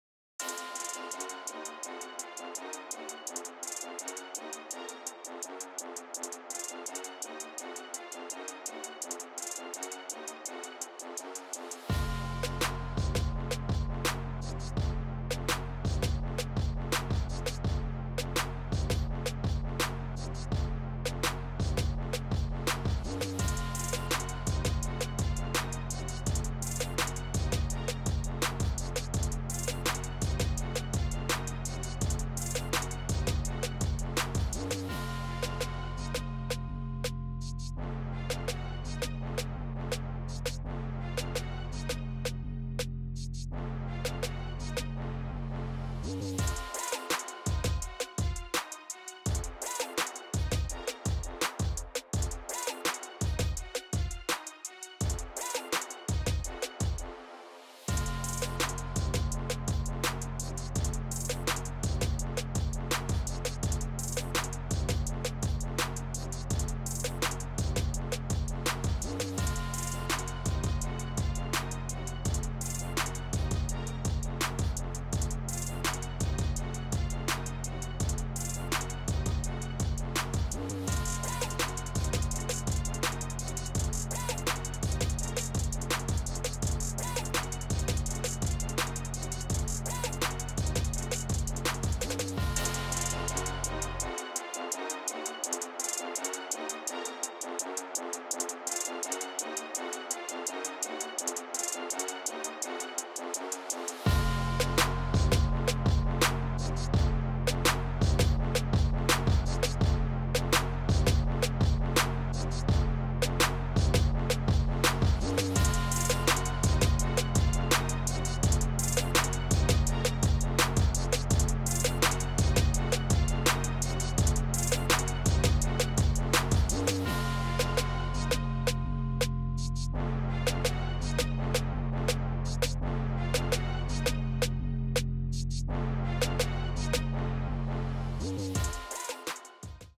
HIGHLIGHTS CALIFORNIA (PA) VS ALDERSON BROADDUS (11-28-22)
THE 18TH RANKED VULCANS HANDLE THE BATTLERS AT HOME FOR A NON-CONFERENCE VICTORY.